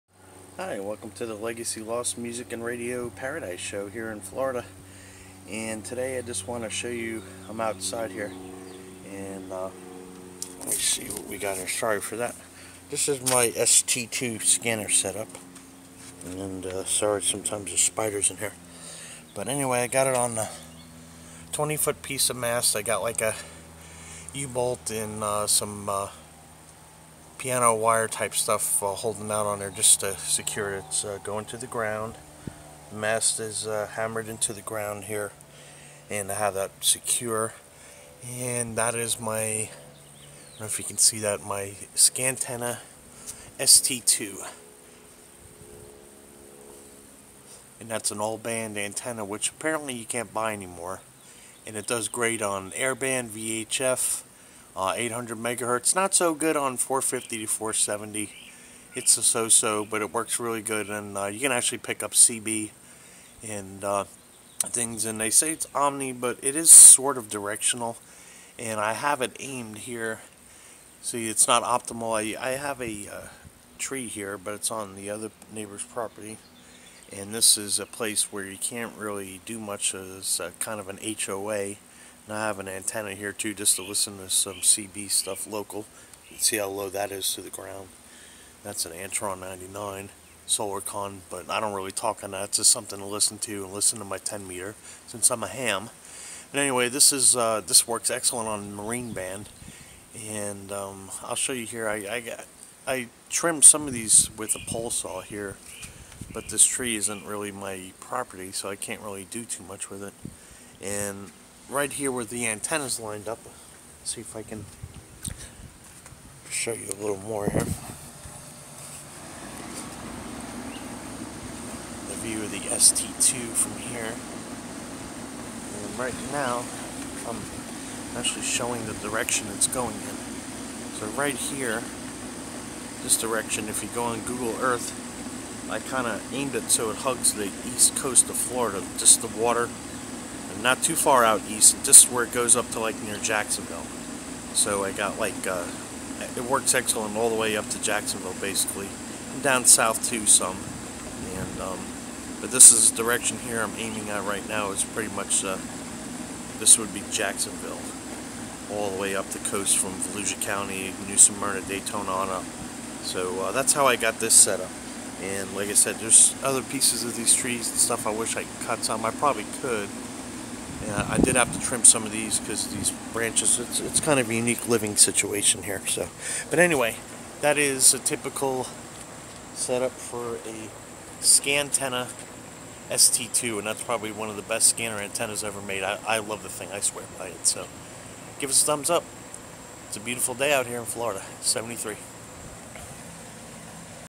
Antennacraft Scantenna ST-2 on 20 foot mast aimed along the FL East Coast
My favorite best performing all band 25-1300 MHz Scanner Antenna the discontinued Scantenna ST-2 75 ohm Antenna-- works excellent all around antenna